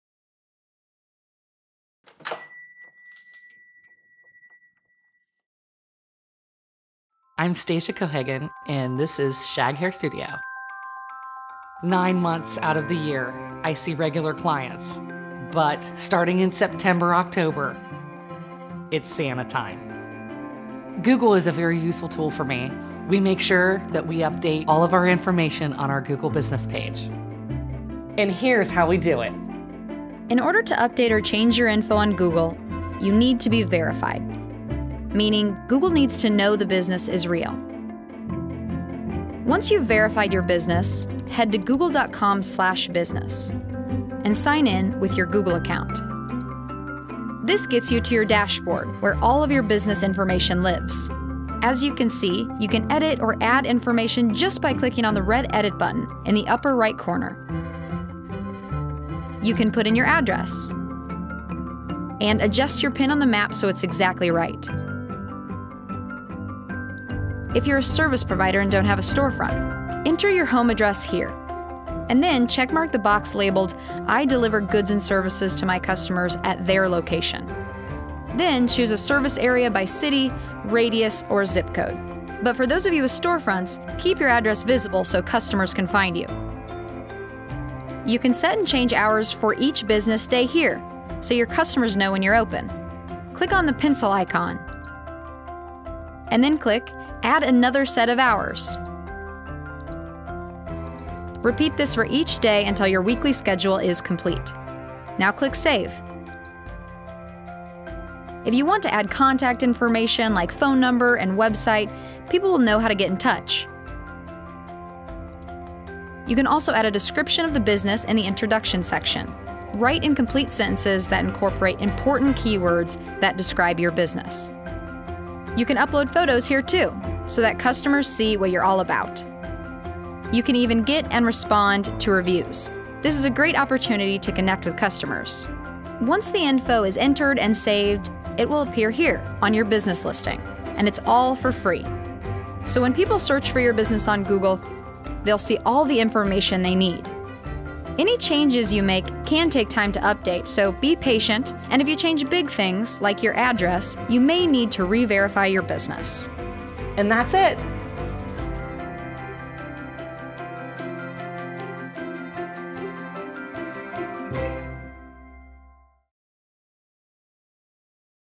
Learn the basics of Google My Business
Once you’ve verified your business on Google, learn the basics of how a Google My Business listing helps you reach customers on Google Search and Maps.